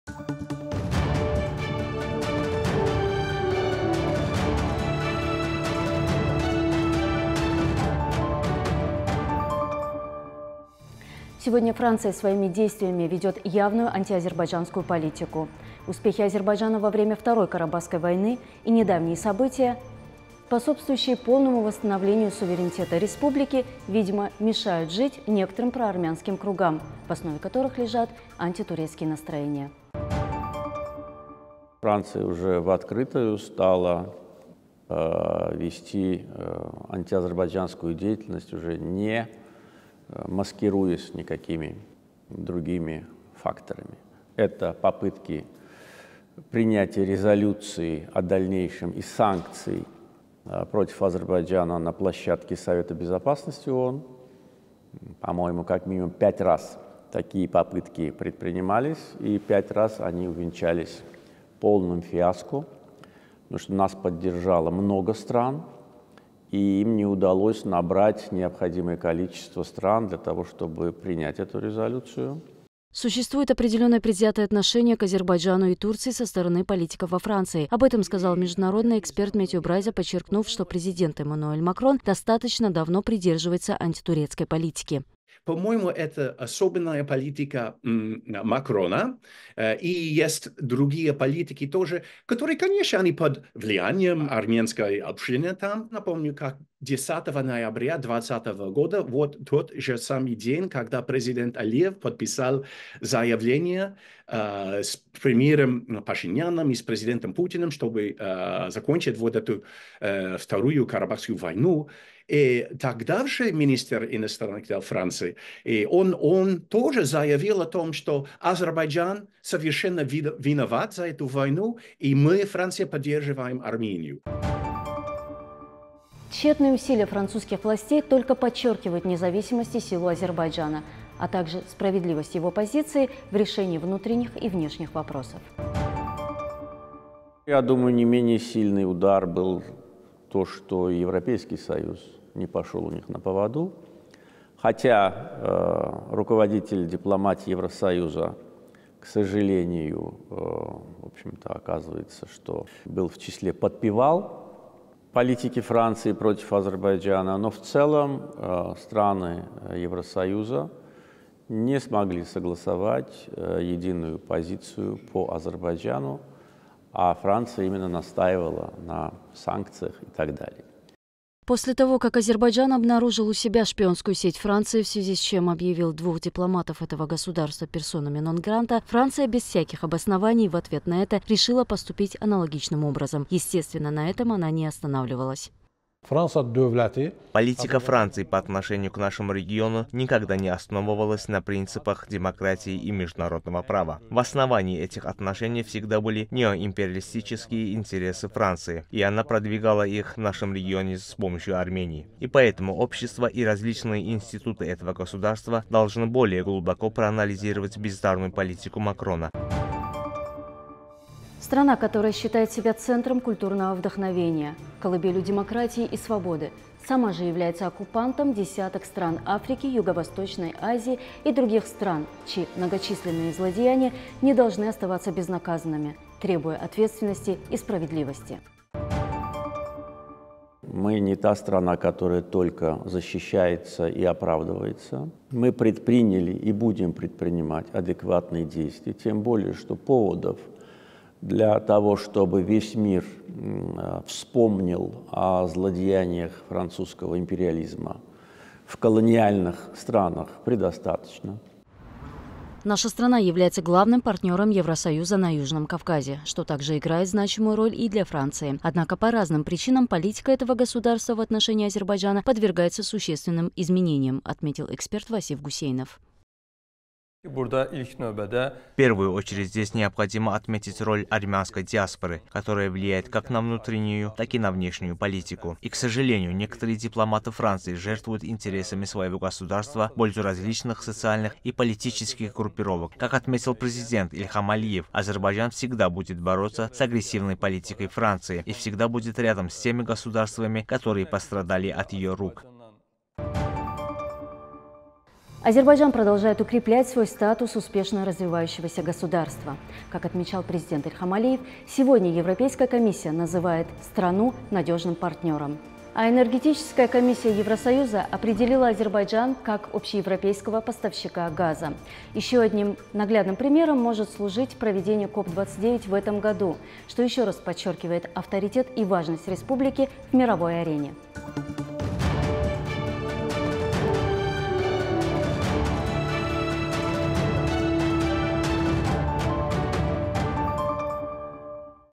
Политика Макрона по отношению к Азербайджану
Сегодня Франция своими действиями выражает явную антиазербайджанскую политику. Успехи Азербайджана во время 2-ой Карабахской войны и недавние события, способствующие полному восстановлению суверенитета Республики, видимо мешают жить некоторым проармянским кругам, в основе которого лежат антитурецкие настроения.